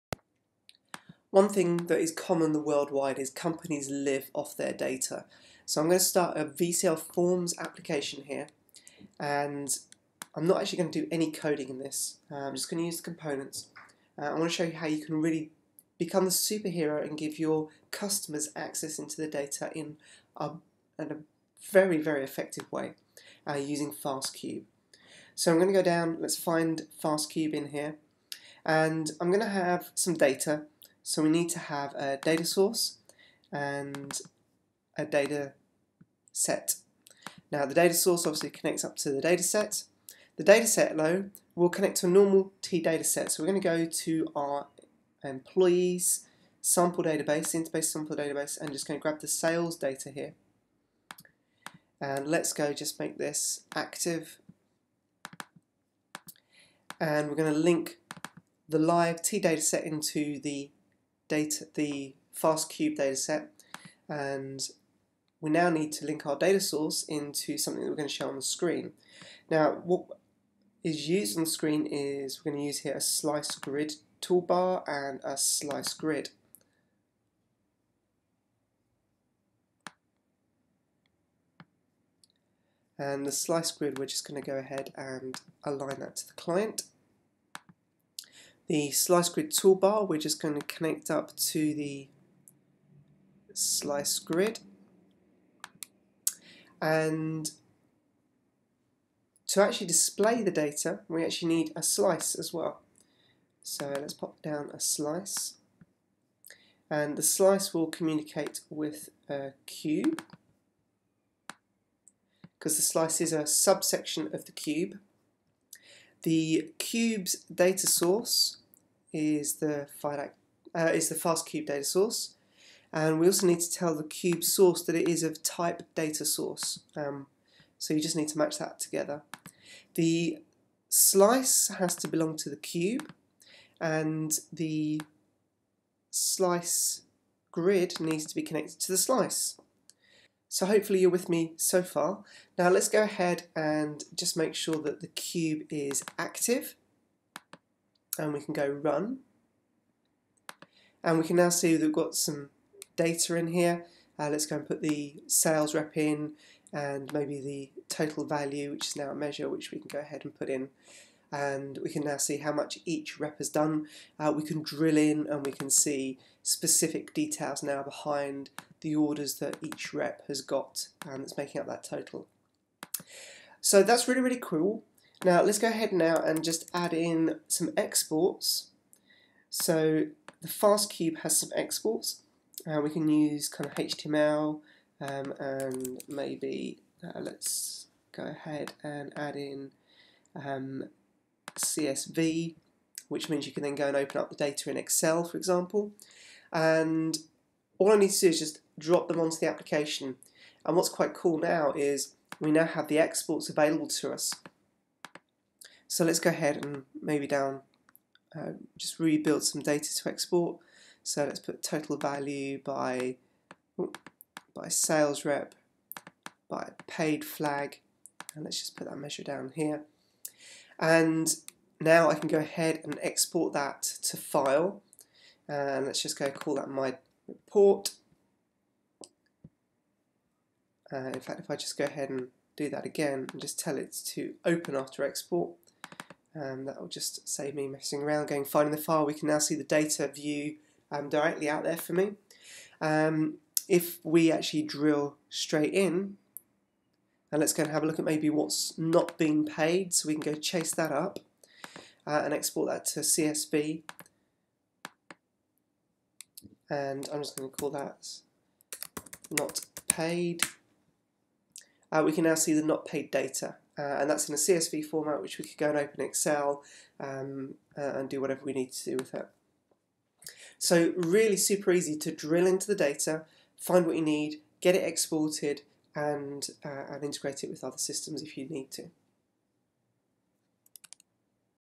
Fast Cube for Delphi
See how fast cube can help you speed up getting to key data. Fast Cube is part of the RAD Solution Pack.